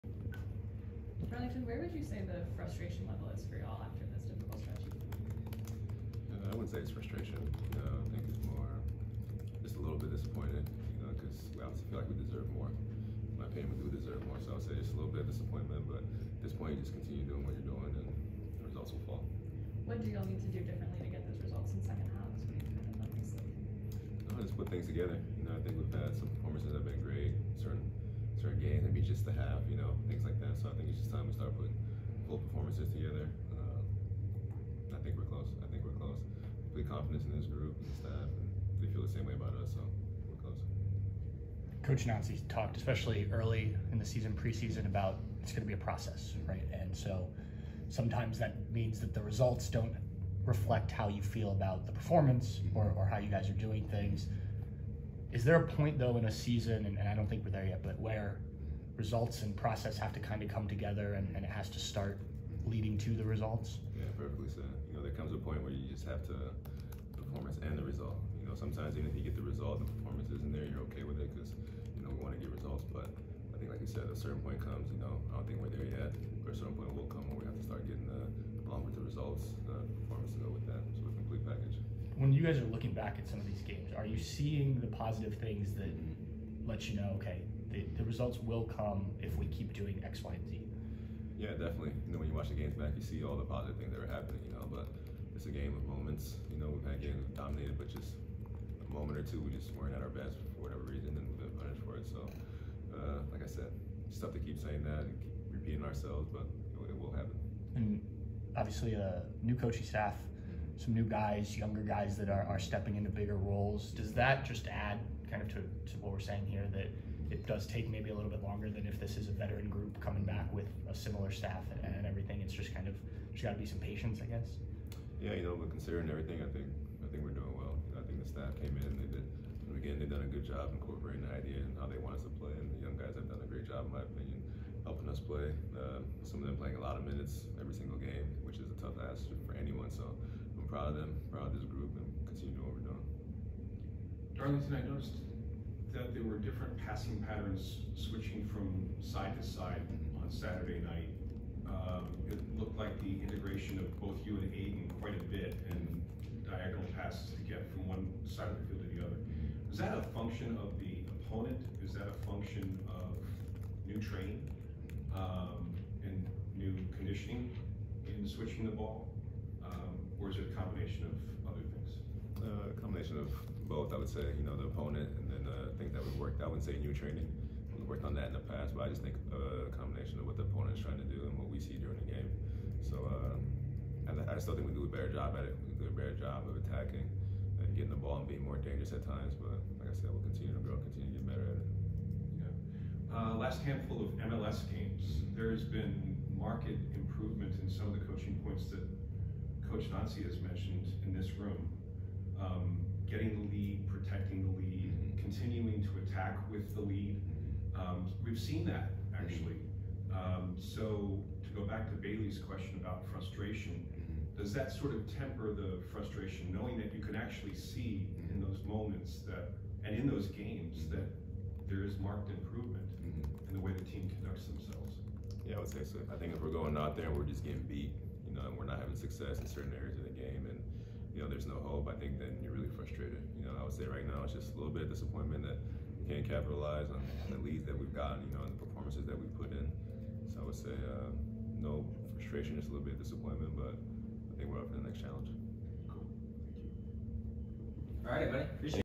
Columbus Crew's Darlington Nagbe on the team's disappointment in recent results
The Black & Gold players believe they should be in a better spot than they are currently in the standings.